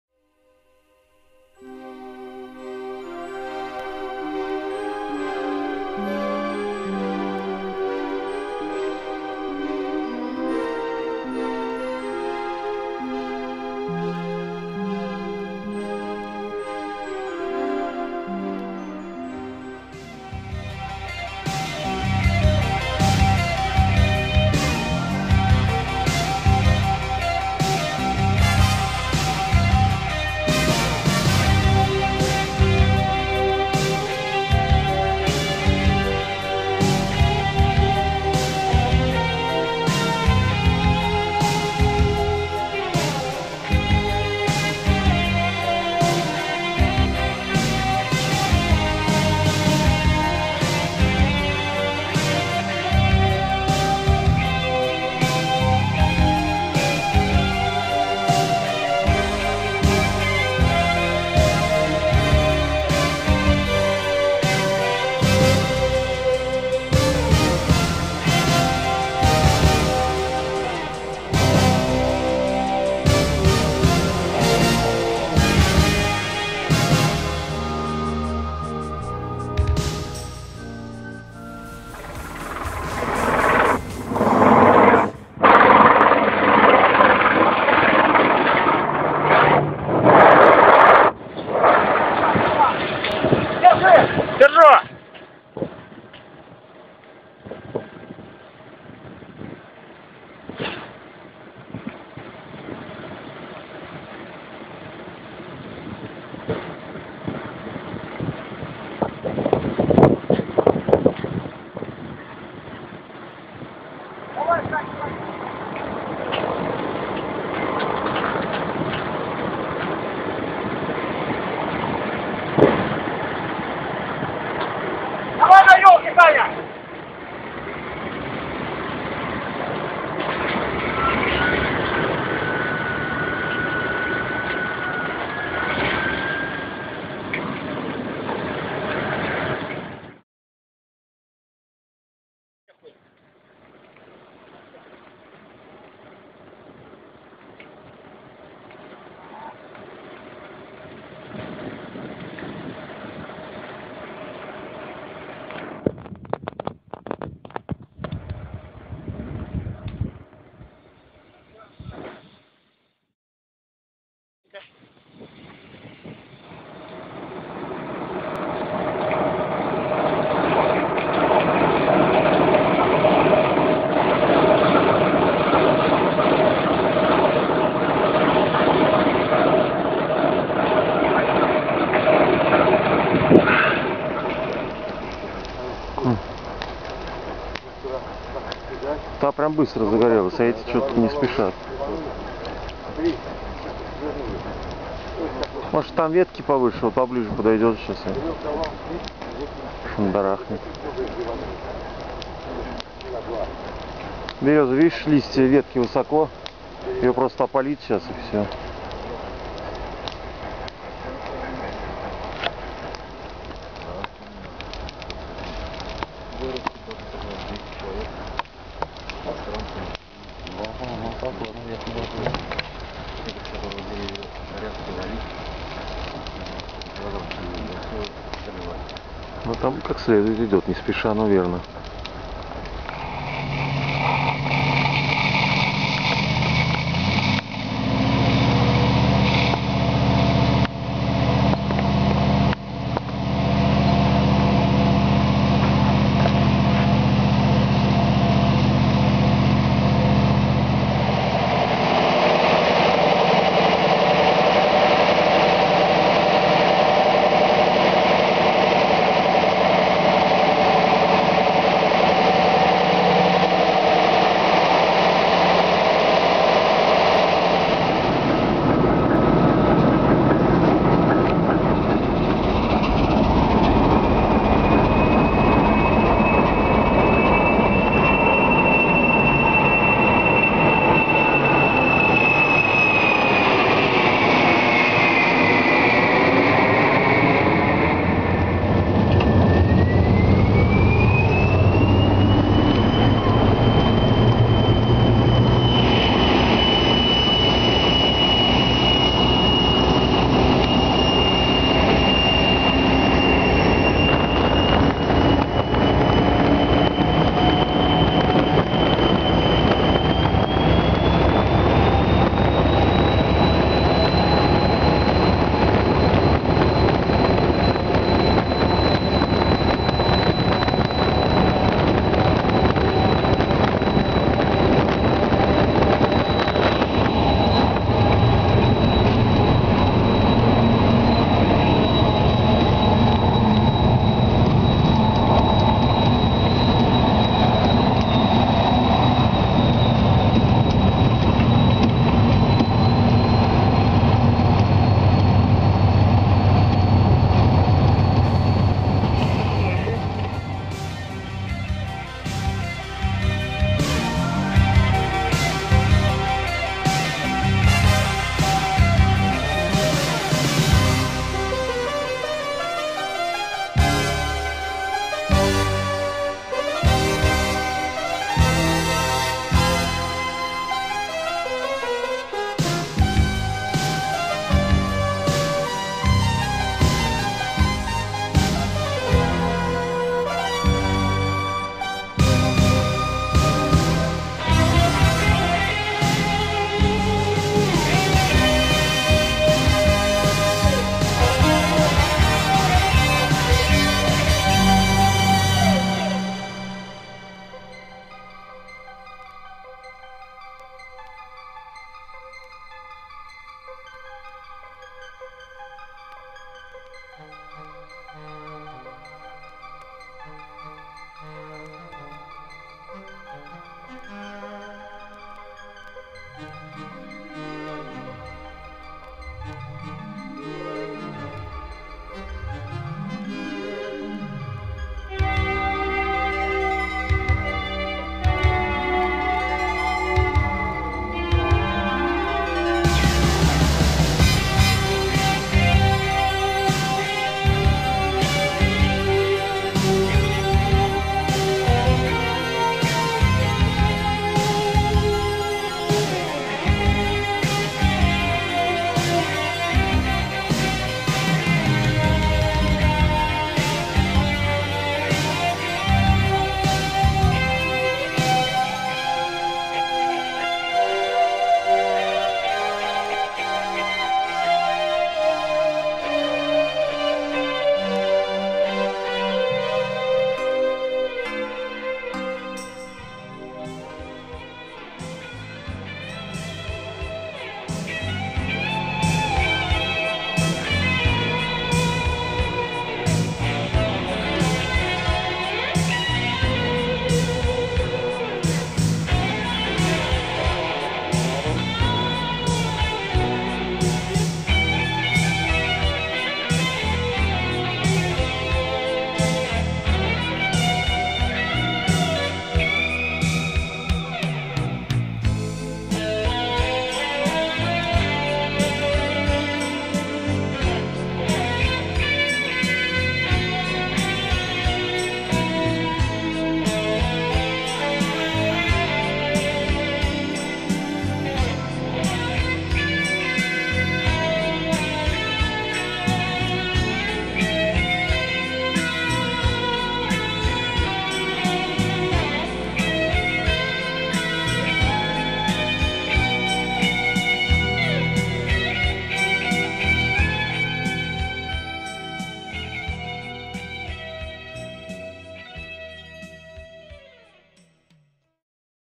Trains and Fires.Поезда и Пожары.
Footage for this video shot in August 2010. During the terrible fires occurred in the central regions of Russia. The very first footage of fires filmed a mobile phone. Therefore the quality of them - bad. But they show the most dramatic events of those fires. Thanks to the selfless, heroic efforts of many people, these fires are stopped.  Это видео снято во время августовских пожаров 2010 года.